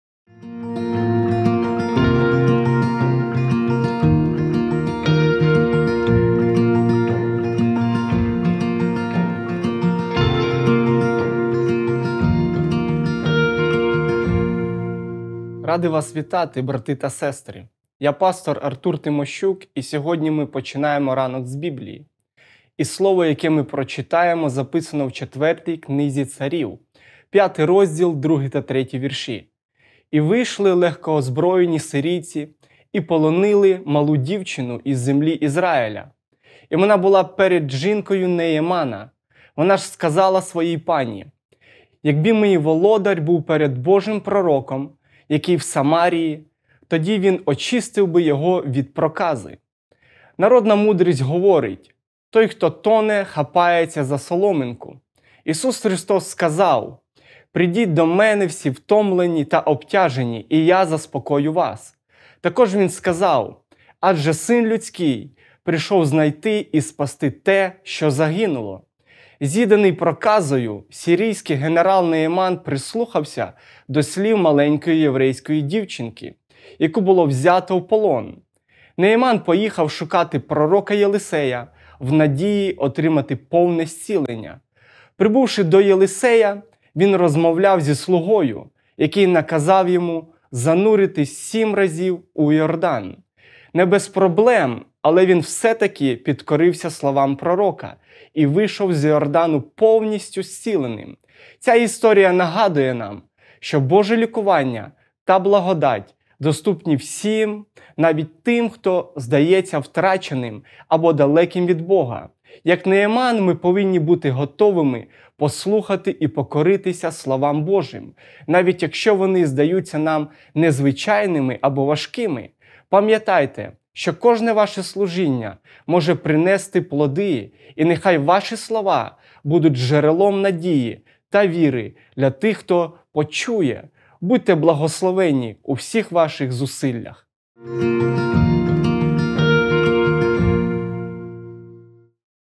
«Щоденний хліб» / 6 травня / Артур Тимощук
«І вийшли легкоозброєні сирійці, і полонили малу дівчину із землі Ізраїля, і вона була перед жінкою Немана. Вона ж сказала своїй пані: Якби мій володар був перед Божим пророком, який в Самарії, тоді він очистив би його від його прокази» 2 Царів 5:2-3 (CUV)